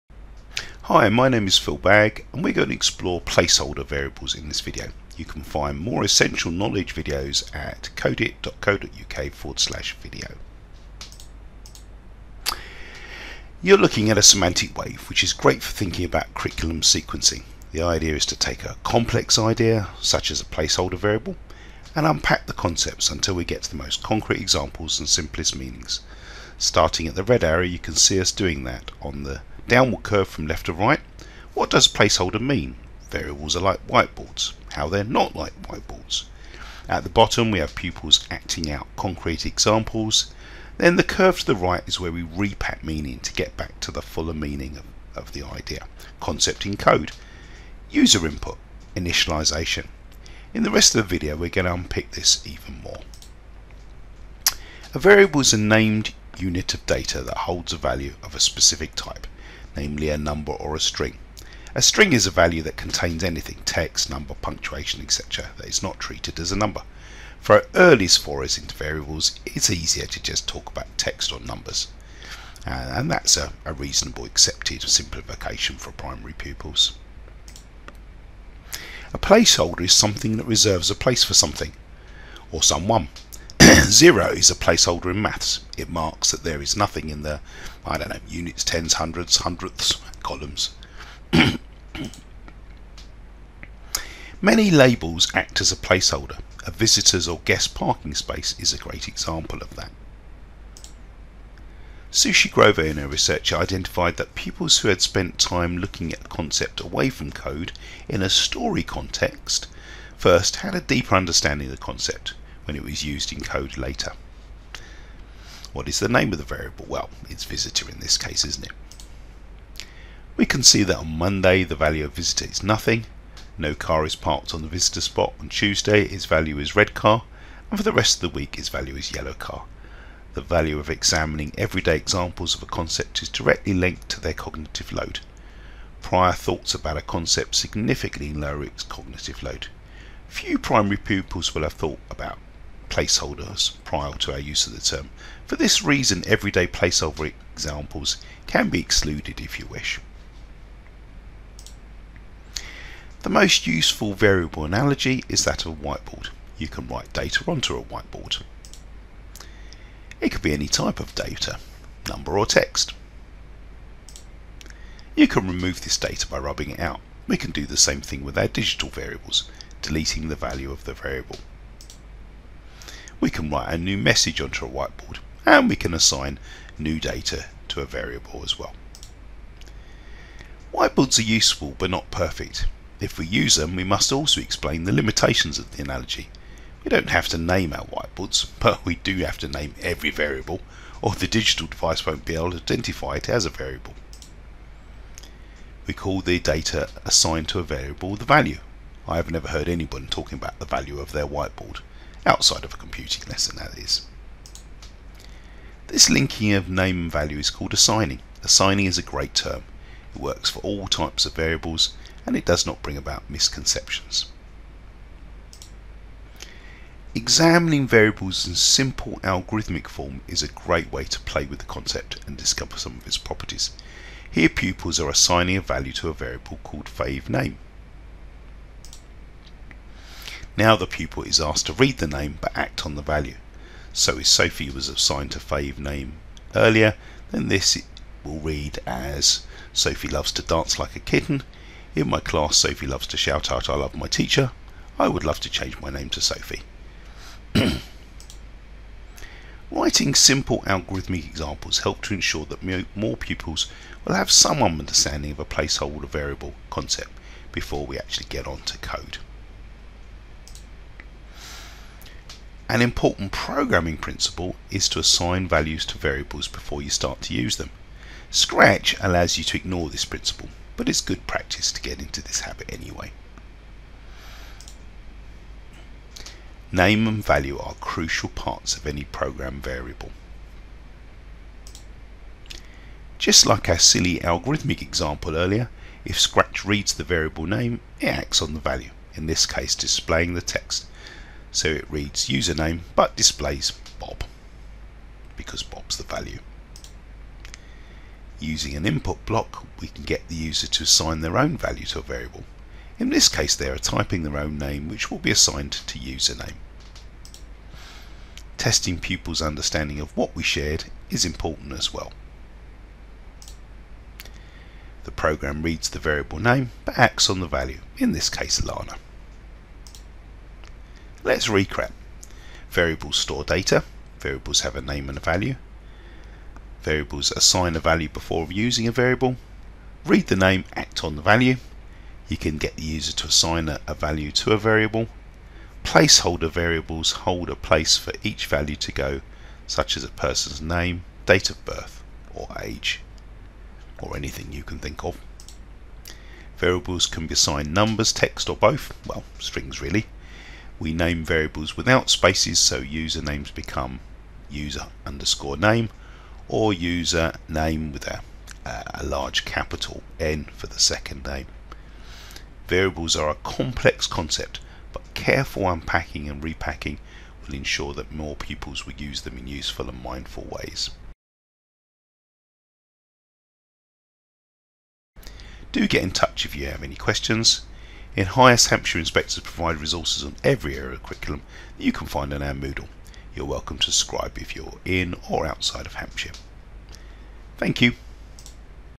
Placeholder Variable Knowledge Video
This video contains way we can introduce a complex concept such as a placeholder variable before we use the concept in code. It is designed for non specialist teachers in KS2 (upper elementary) or KS3 (lower secondary)